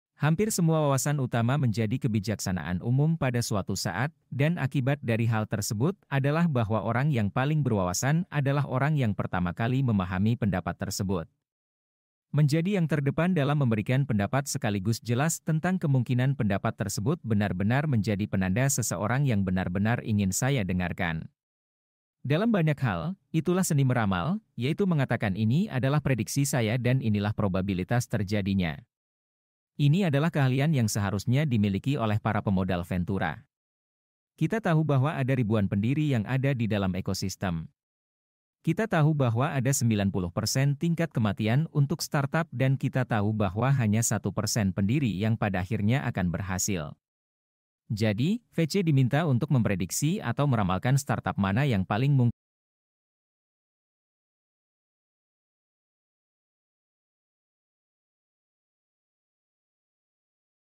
Apa inti dari bisnis modal ventura?
"Wawasan utama yang saya kumpulkan adalah bahwa hampir semua wawasan pada akhirnya menjadi kebijaksanaan umum, dan mereka yang benar-benar berwawasan adalah mereka yang memahami ide-ide ini terlebih dahulu. Menjadi yang terdepan dalam sebuah opini dan memahami kemungkinannya adalah ciri khas seseorang yang layak untuk didengarkan. Hal ini merangkum seni meramalkan-membuat prediksi dan menilai kemungkinannya. Ini adalah keterampilan penting bagi VC, yang harus menavigasi ekosistem yang dipenuhi dengan ribuan pendiri, mengetahui bahwa hanya sebagian kecil yang akan berhasil. Dengan tingkat kegagalan 90% untuk startup, VC ditugaskan untuk memprediksi usaha mana yang akan berkembang jauh sebelum mereka melakukannya. Keberhasilan di arena ini memberi mereka penghargaan, sementara prediksi yang meleset sering kali mengundang cemoohan. Pada akhirnya, inti dari bisnis ini terletak pada keakuratan prediksi ini." - Jeremy Au, Pembawa Acara BRAVE Southeast Asia Tech Podcast

观看、收听或阅读全文，请访问 ve-secrets-cn

Xem, nghe hoặc đọc toàn bộ thông tin chi tiết tại